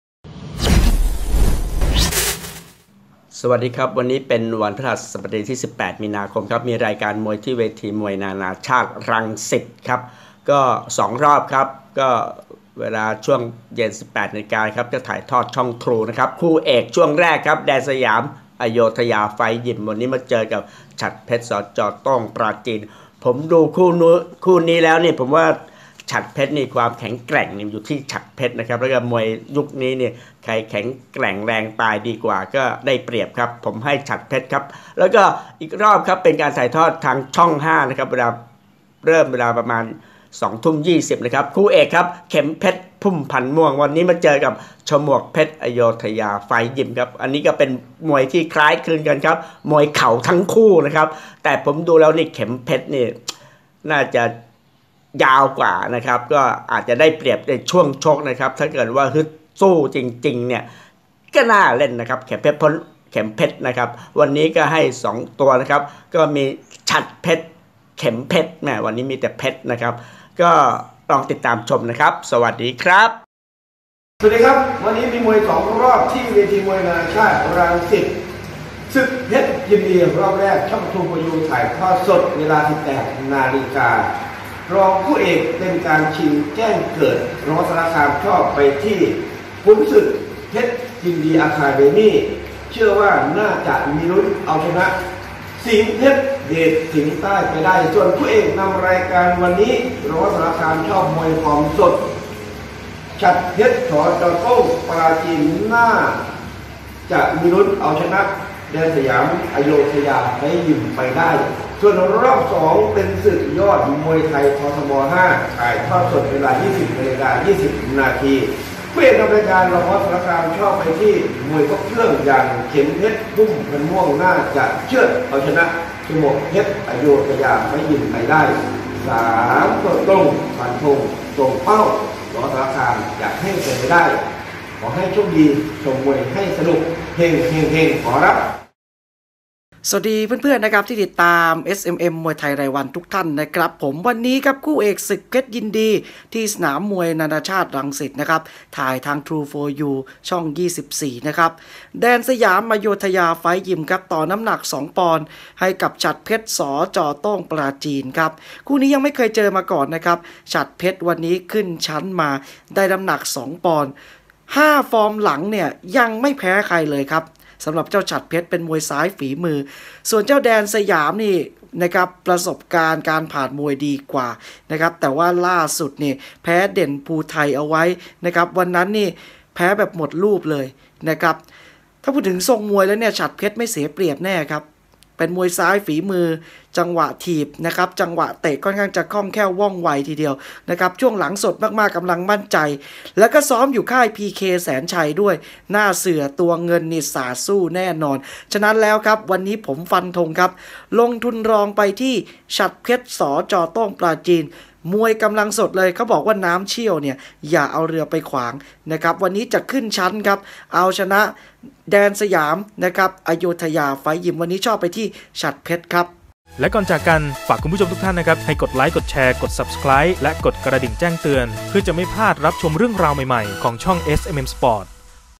ทัศนะมวยไทย | ศึกเพชรยินดี 18 มี.ค. 64 คู่เอก แดนสยาม อโยธยาไฟต์ยิม พบ ฉัตรเพชร สจ.โต้งปราจีน
ทัศนะมวยไทย | วันพฤหัสบดีที่ 18 มีนาคม 2564  ที่เวทีมวยนานาชาติรังสิต
- ศึกเพชรยินดี คู่เอก แดนสยาม อโยธยาไฟต์ยิม พบ ฉัตรเพชร สจ.โต้งปราจีน
- ศึกยอดมวยไทย ททบ.5 คู่เอก เข็มเพชร พุ่มพันธุ์ม่วง พบ ฉมวกเพชร อโยธยาไฟต์ยิม